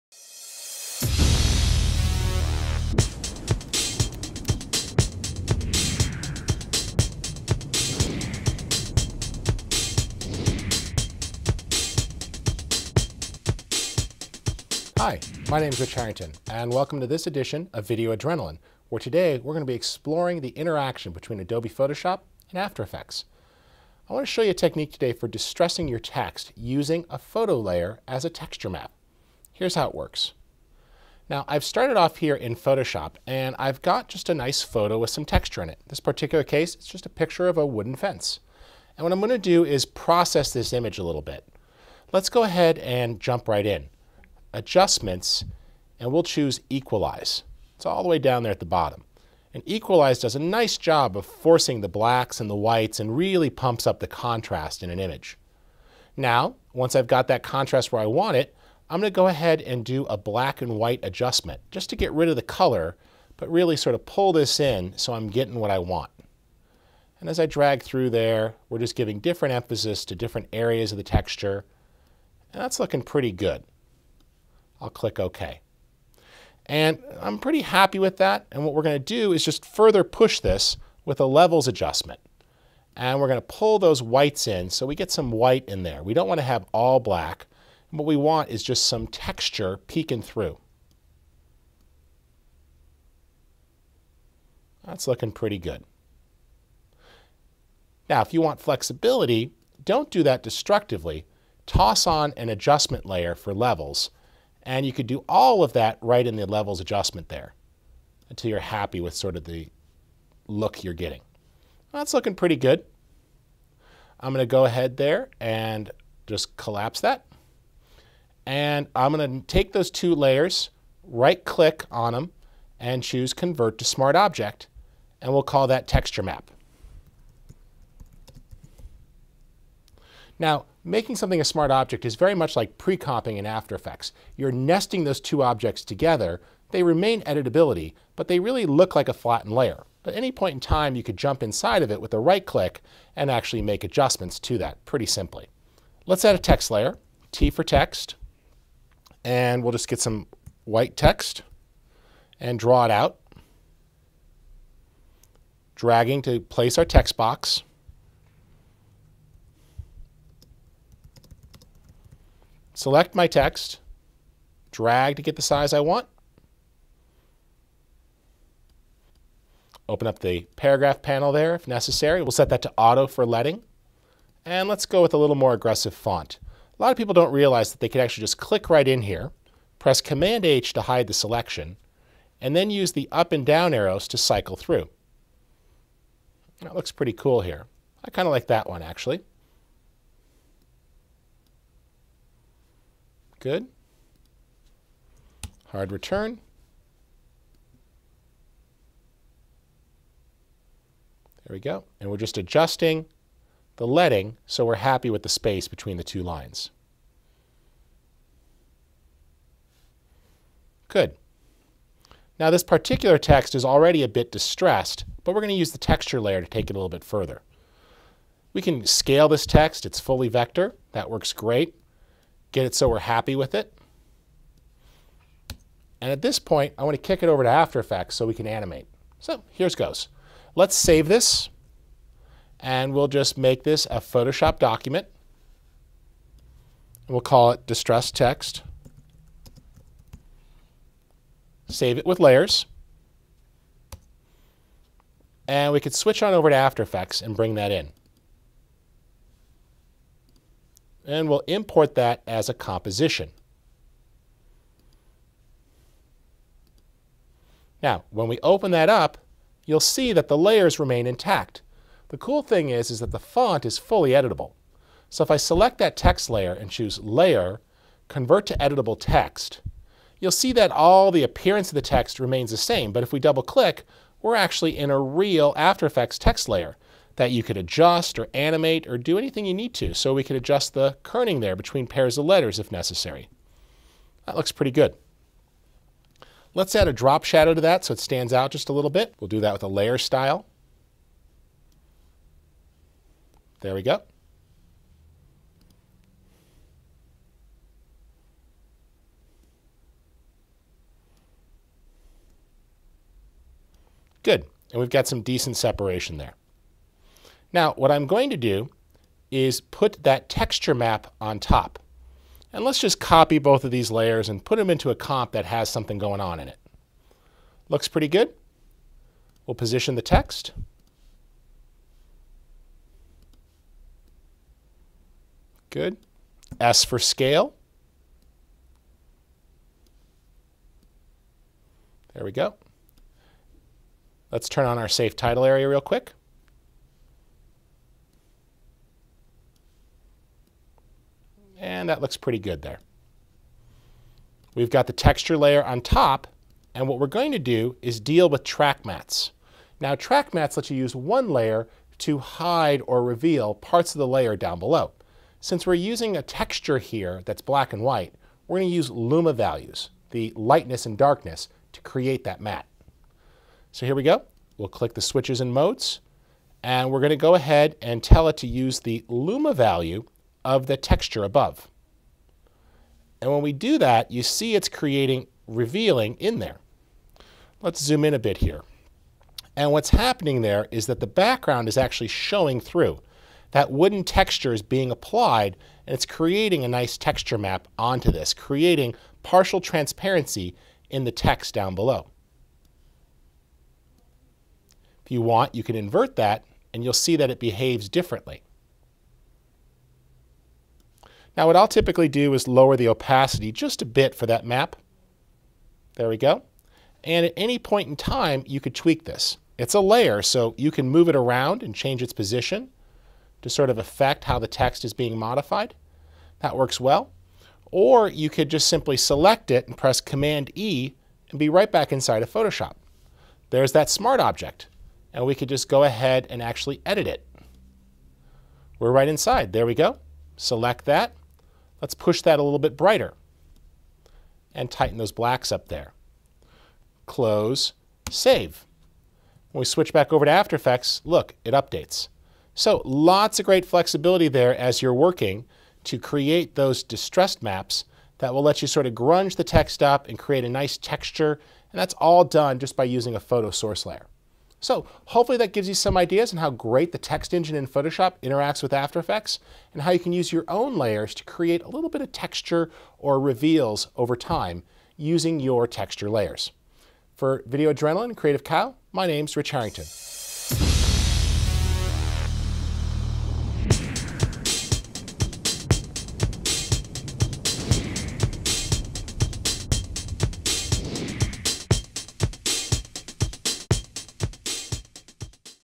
Photoshop and AE: Distressed Text
In this video tutorial Richard Harrington shows a technique for distressing text using a photo layer as a texture map. Richard starts the project in Photoshop and finishes working with the layers in After Effects for the final touches.